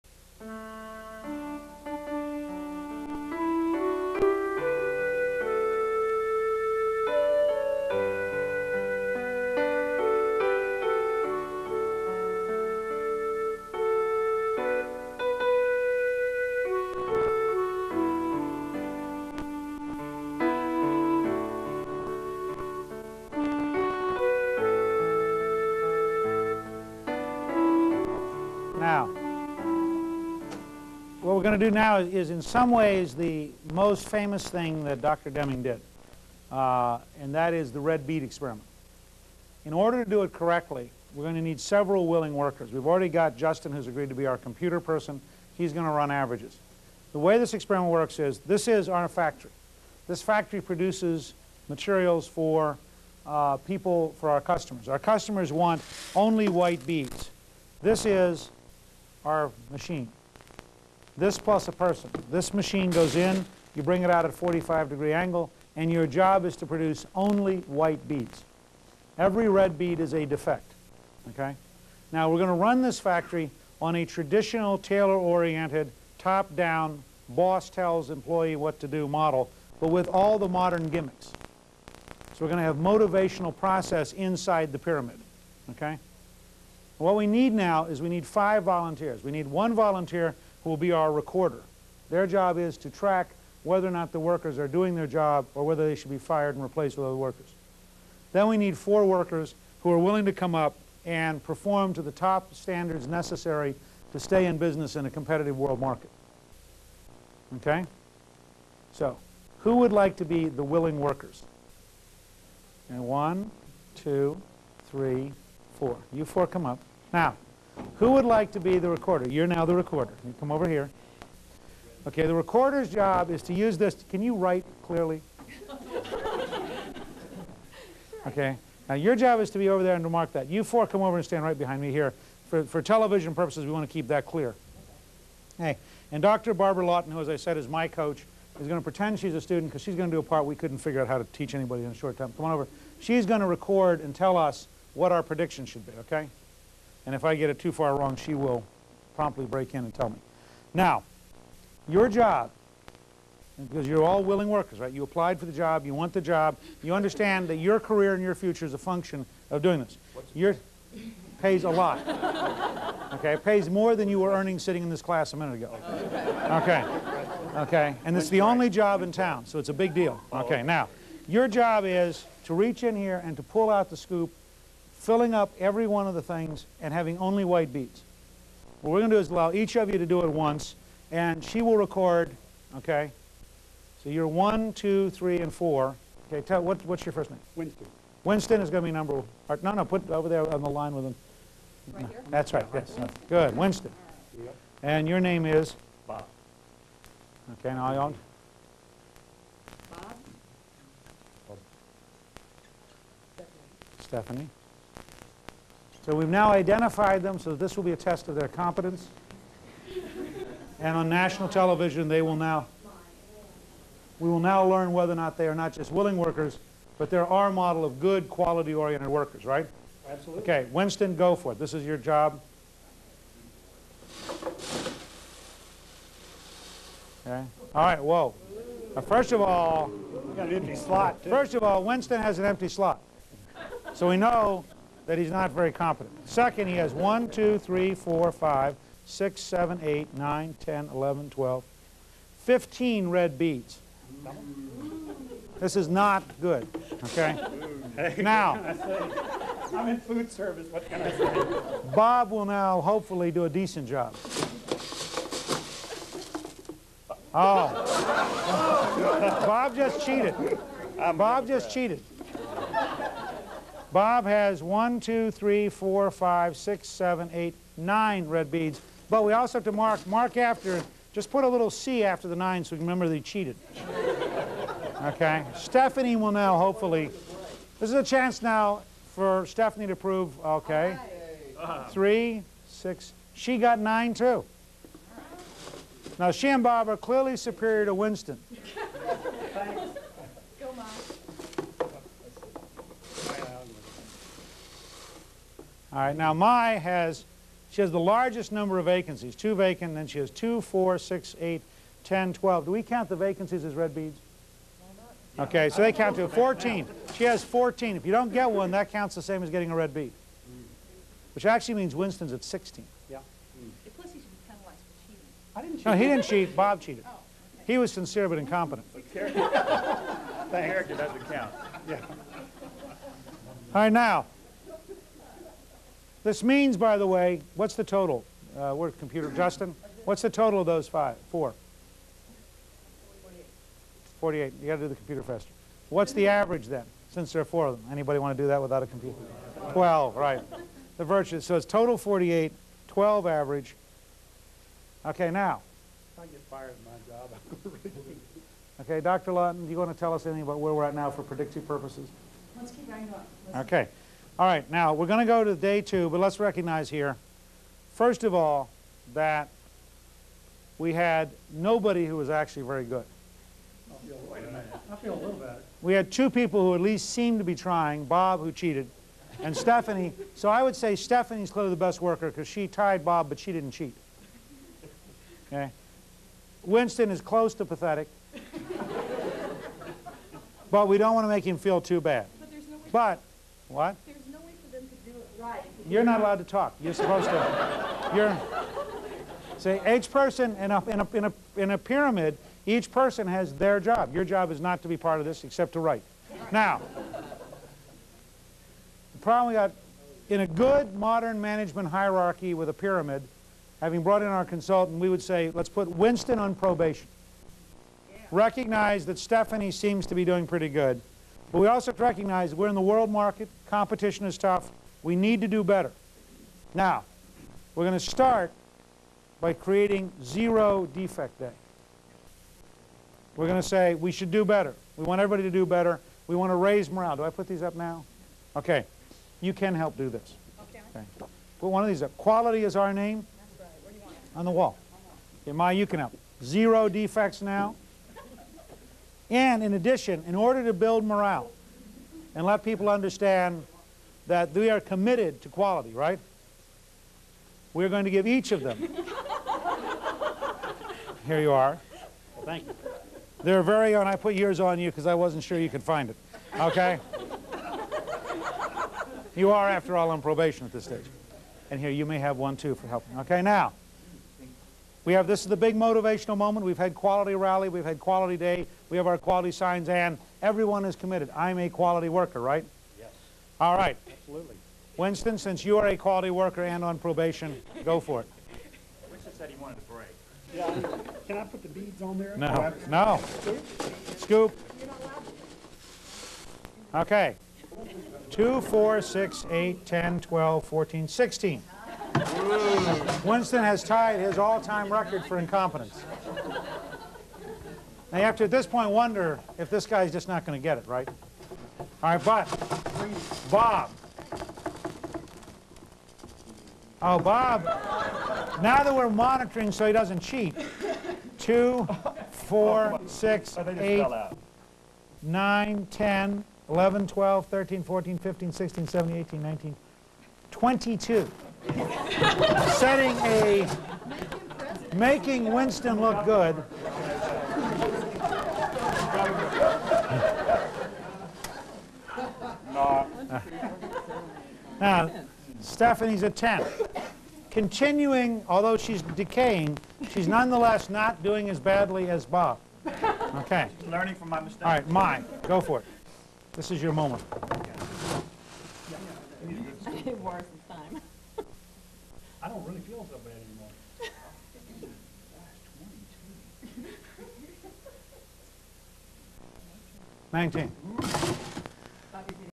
RAC06g.mpg
Renewing American Civilization Course - Lecture Six -- February 11, 1995 -- Pillar Five, Quality as Defined by Deming (Part 07 of 12)